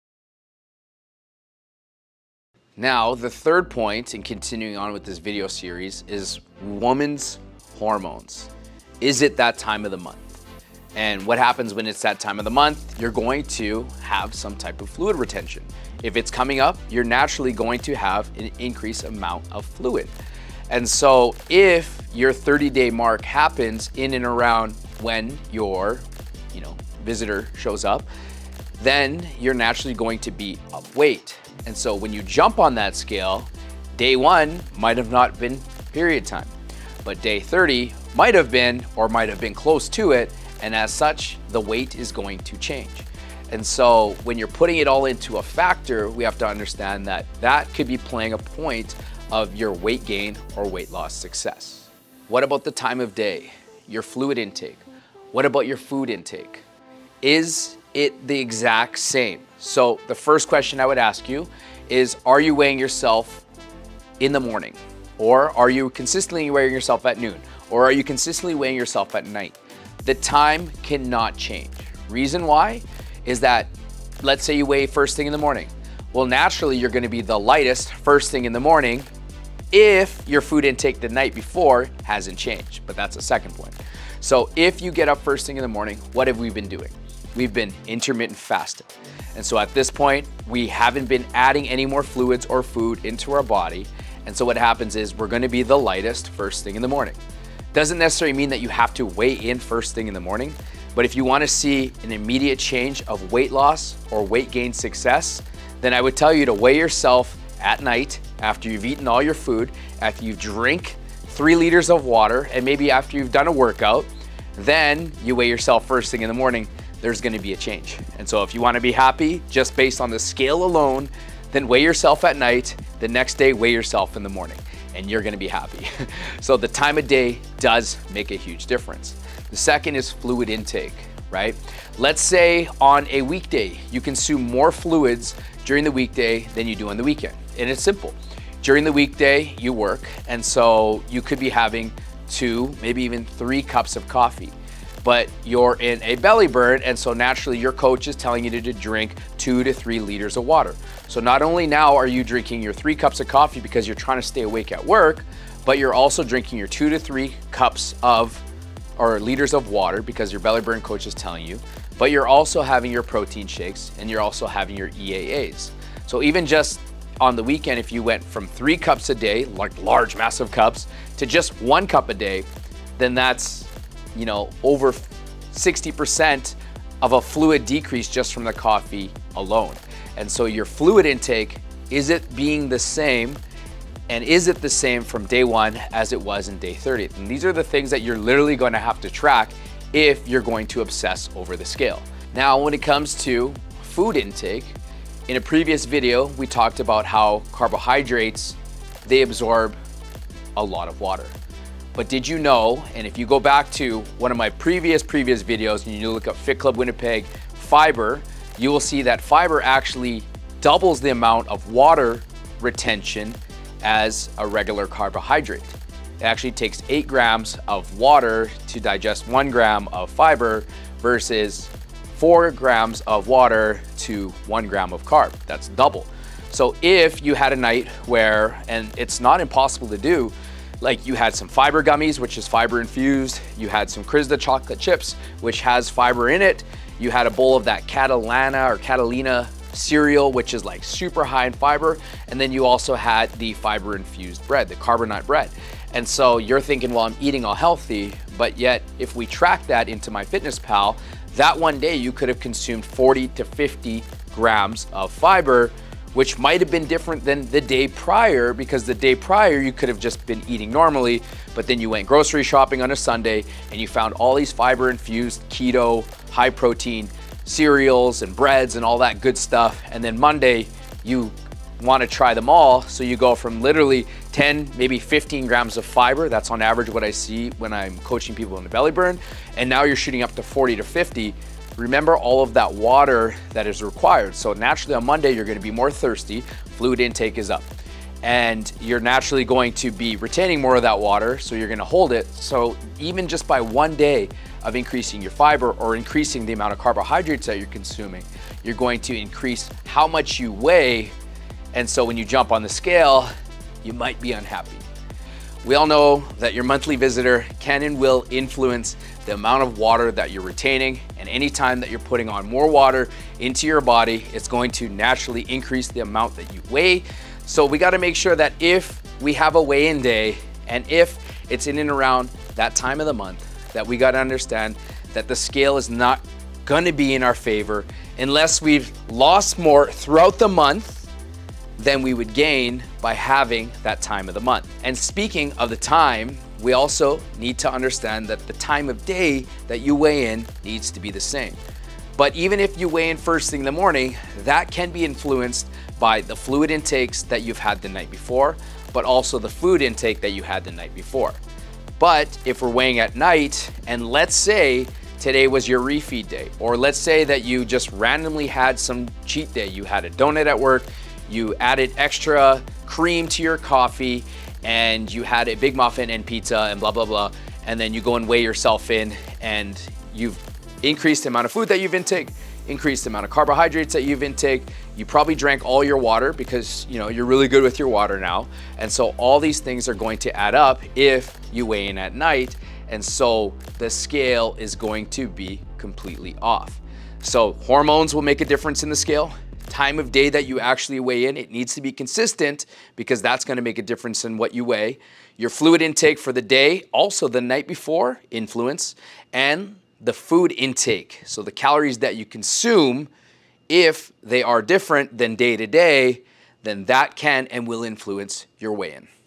Secret thing that makes you gain weight! Series 3
🔥🚀 Unlock the Secret Thing that Makes You Gain Weight! 🤯 Series 3 is HERE! Ready to dive into the ultimate weight loss revelation? Brace yourselves! 🏋️‍♂️💥

🔍 Ever wondered why the scale refuses to budge despite your efforts? 🤔 It's time to uncover the mystery behind those stubborn pounds! 🕵️‍♀️

👀 Discover the game-changing insights about hormones, especially during the period time for women! 💁‍♀️💊 Plus, explore the impact of timing – from fluid intake to food consumption. ⏰🥤🍽️ Were they consistent or causing havoc on your progress? 🤷‍♂️

🚀 Let's ignite that fat-burning furnace together! 💬💪 Let's engage and empower each other! 💬💬